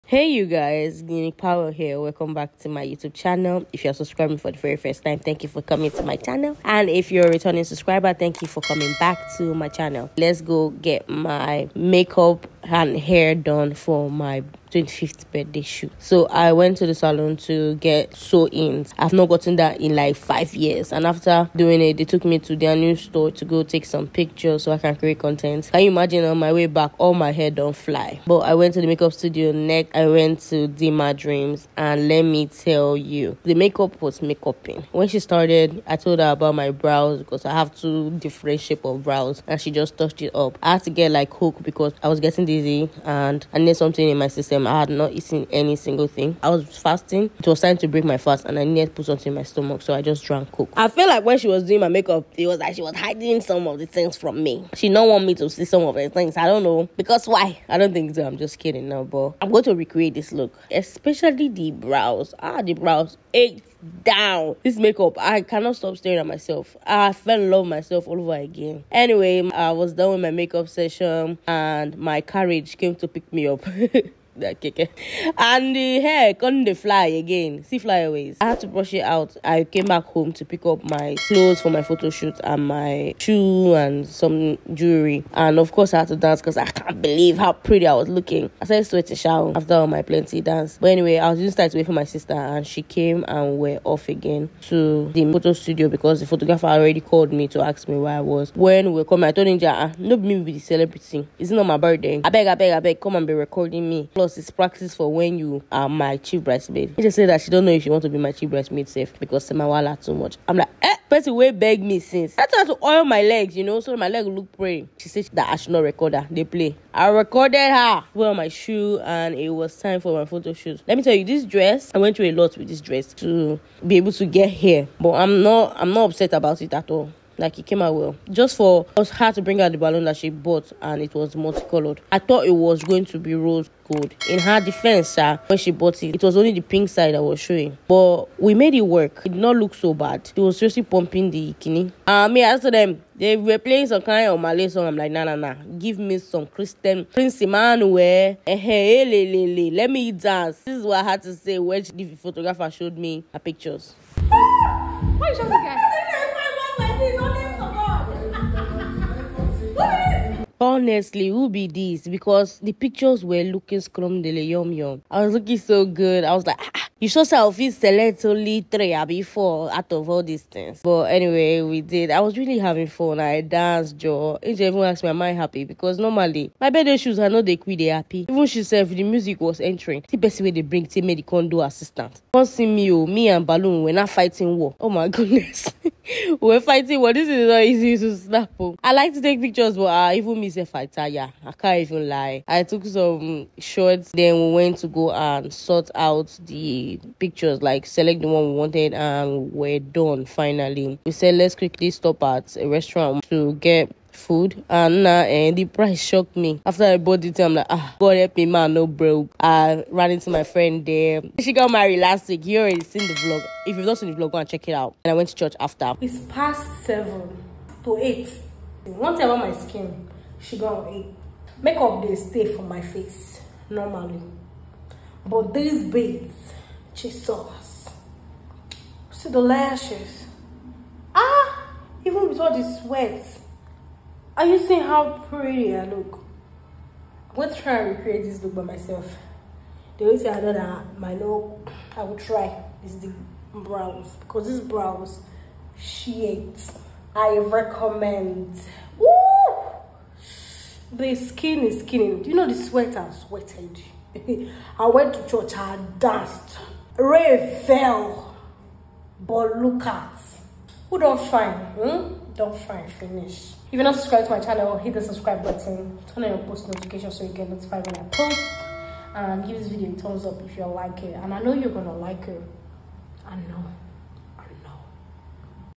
Birthday Vlog: My 25th Birthday Photo Shoot + My first ever Professional Makeup Session
Hey you guys, I know I owe you another story time video but I can explain, my birthday is in a few days and I have been occupied with planning my shoot and everything else

Anyway, I got my very first professional makeup done and honestly I’m in love, I’m glad that the Holy Spirit laid it in my heart (last minute) to get my makeup done by a professional

I can’t wait for my actual birthday 🎂 🍾🎊